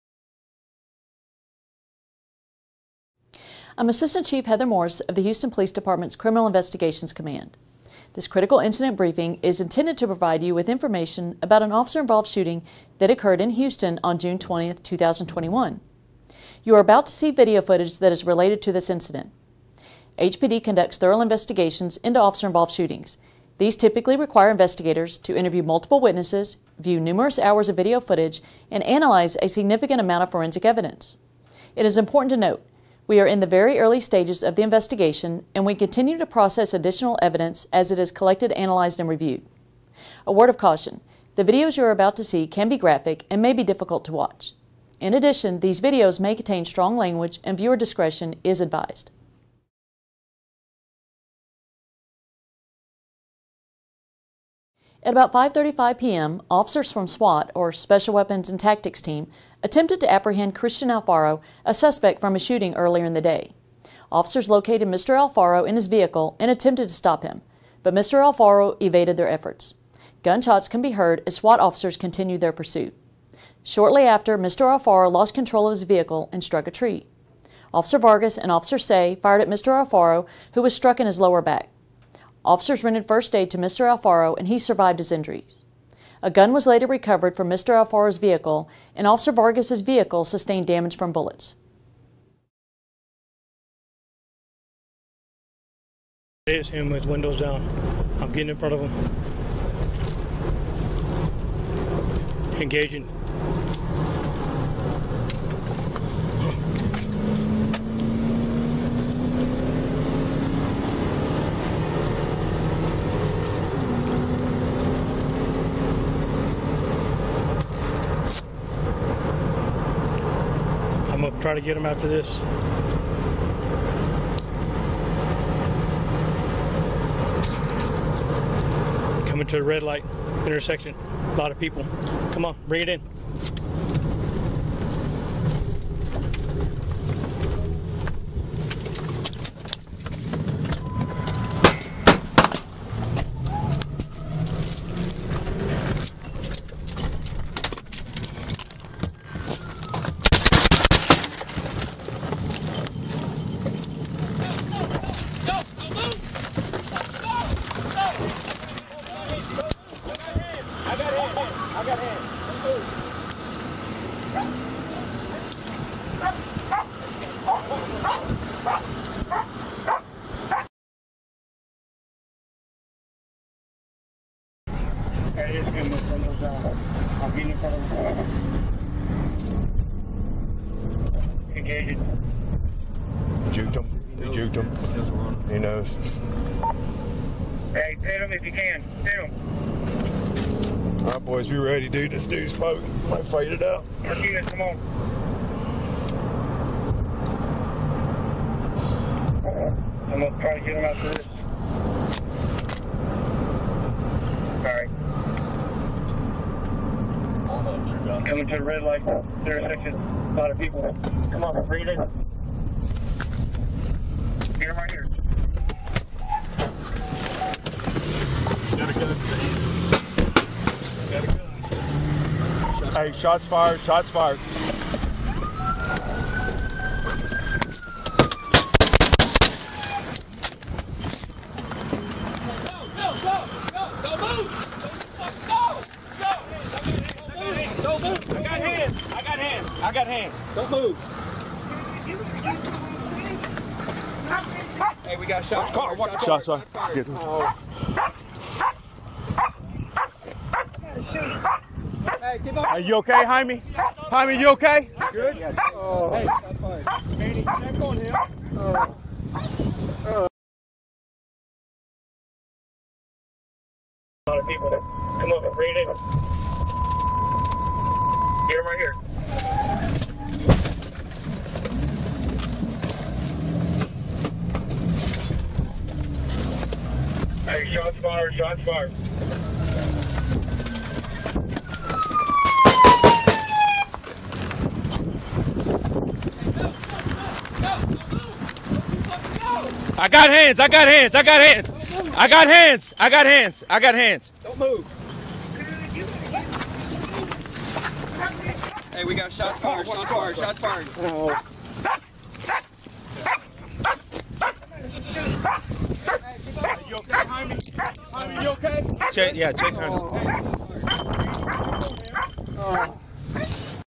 Houston PD 6/20/21 - SWAT officers involved in rolling shootout
**Grab some of the industry leading EDC items over at @vdevelopmentgroup / vdevgru.com (links in profile) and support us!!**

Check out the full video on our YouTube Officer Involved!